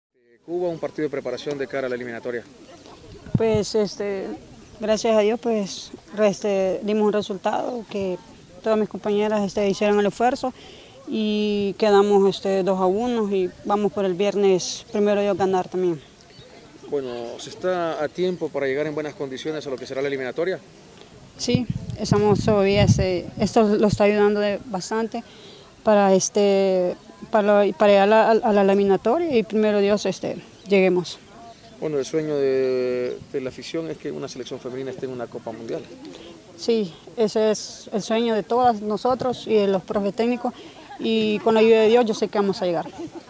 Jeimy Astrid Prudencio - Seleccionada Femenina Mayor
Estos partidos nos están ayudando bastante para llegar de la mejor manera a la eliminatoria